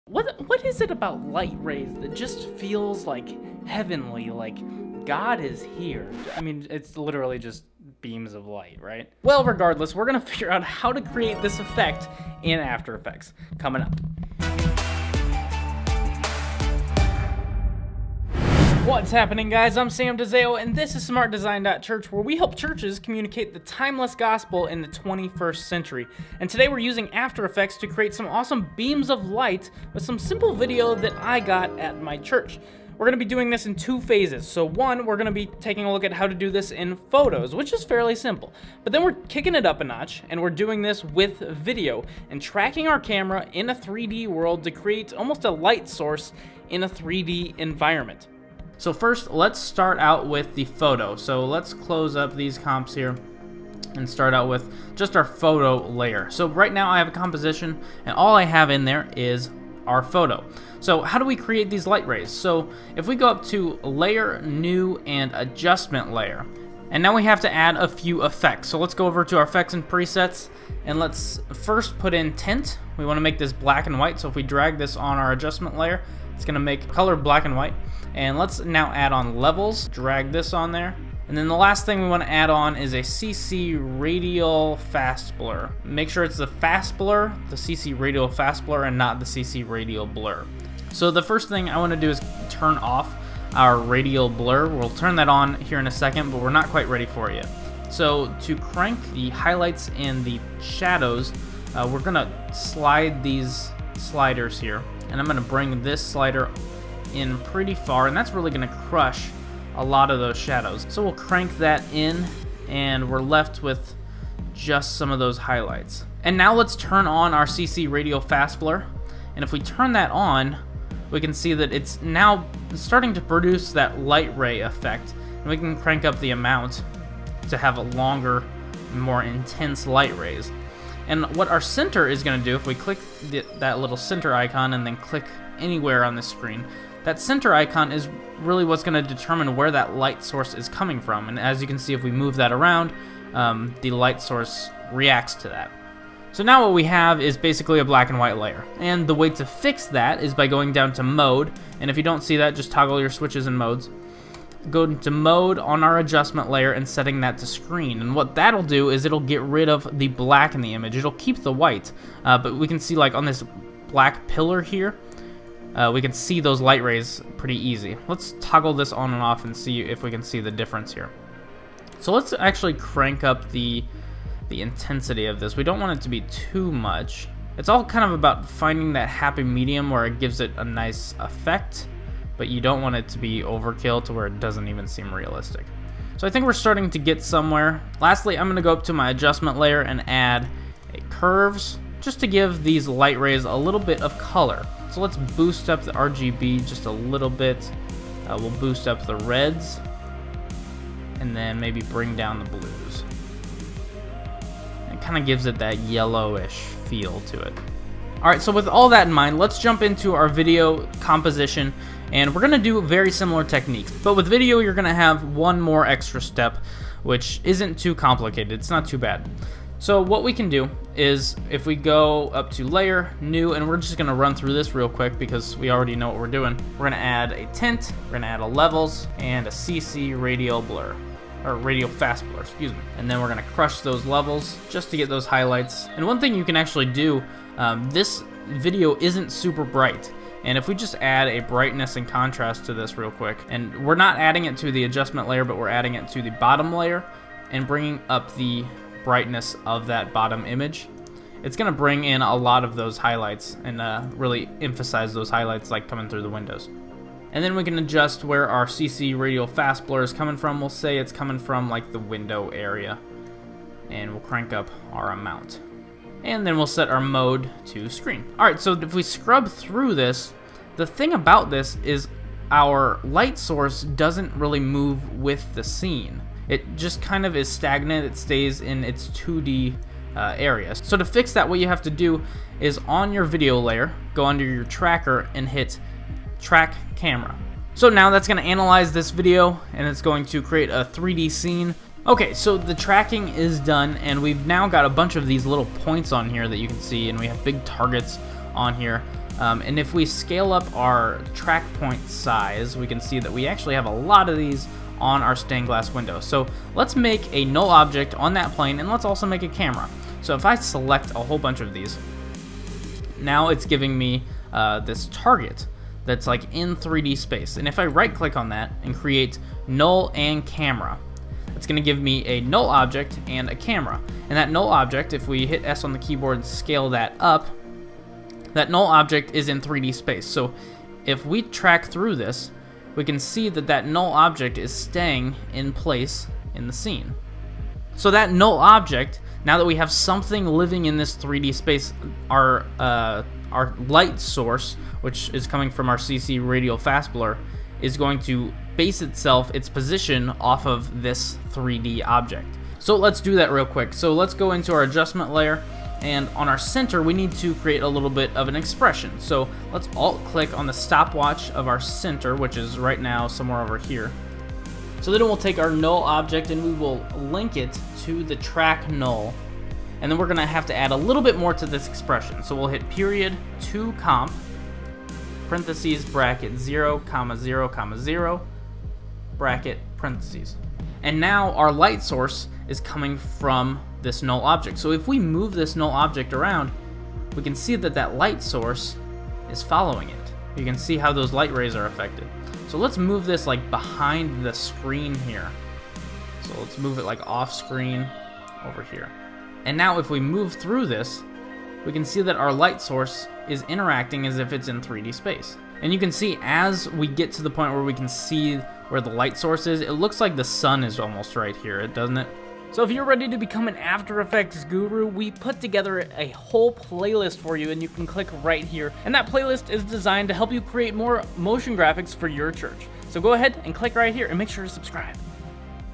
Create Heavenly God Light Rays in After Effects
Since we associate beams of light with a "heavenly" feeling I figured I'd put together a quick tutorial on how to fake that "God Light Rays" effect in After Effects! It's actually super easy!

Thanks to the Patrons of this channel:
Jake Wetterlin